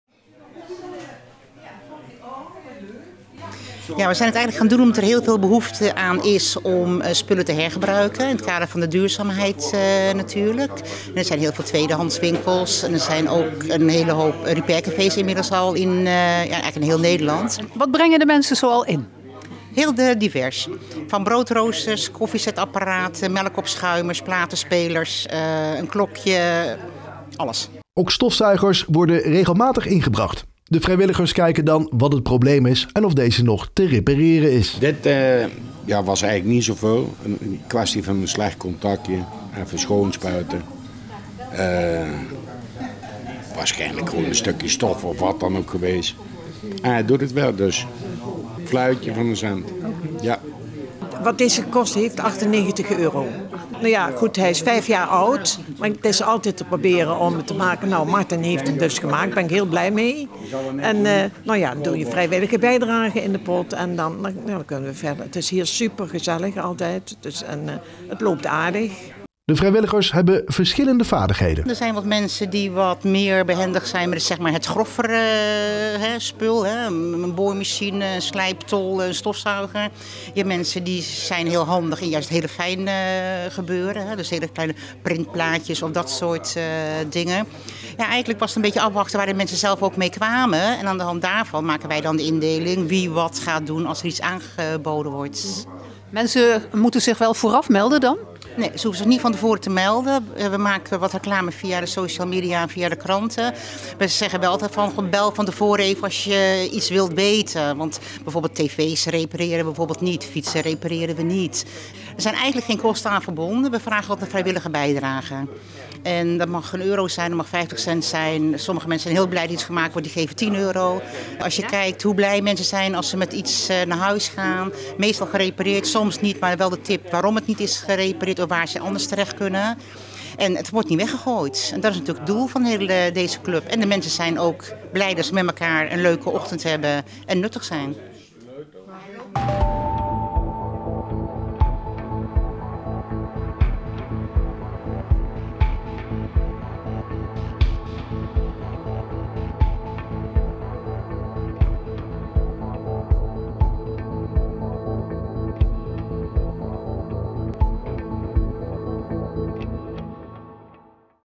Laat spullen repareren in plaats van weggooien || RN7 REGIONIEUWS
Spullen weggooien omdat ze kapot zijn doen we teveel. De afvalberg groeit veel te hard, de omloopsnelhied van spullen is kort en niet duurzaam. Repaircafé’s zijn hier een antwoord op. Alleen in Nijmegen zijn er al zeven. Hier kunnen kapotte spullen naartoe worden gebracht en deze worden door vrijwilligers gerepareerd.